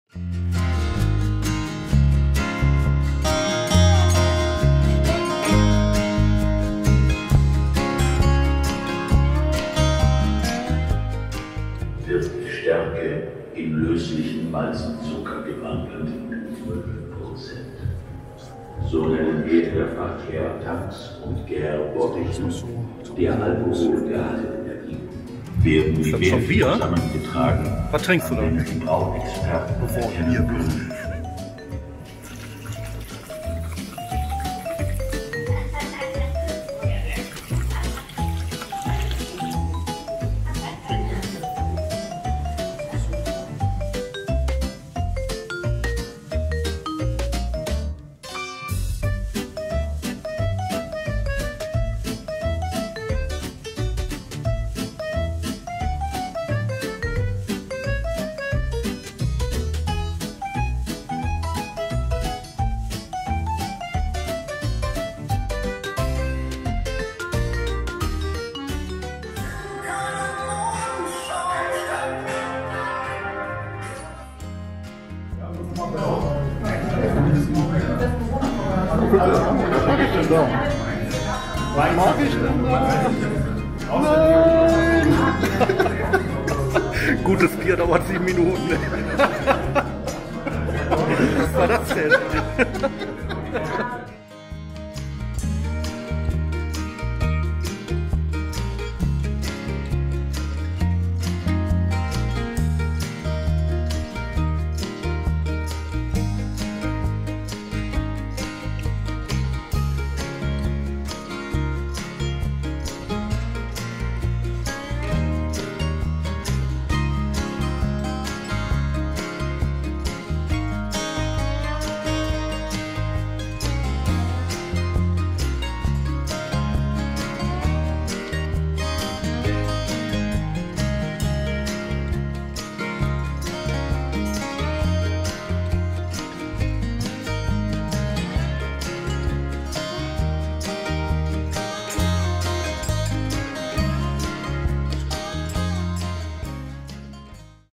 Mit @wanderverliebt in der Brauerei Moritz Fiege in Bochum | POTT.einander |2023
[Unbez./unbeauftr. Werbung]

Ein toller Abend in der MORITZ FIEGE BRAUEREI mit "WanderVerliebt" im Rahmen der BrauKultTour 2023.

Diese Brauereiführung ist wärmstens zu empfehlen. Die Brauprozesse werden anschaulich erklärt und man bekommt einmalige Einblicke in das heutige Handwerk des bierbrauens.

Voller Lokalkolorit geht es auch schließlich zur Verkostung unseres Lieblingsgetränks...Mehrere Geschmacksproben verschiedener Biersorten des Hauses dürfen genossen werden...Natürlich stilecht mit der "echten" und einzig wahren Currywurst aus Bochum...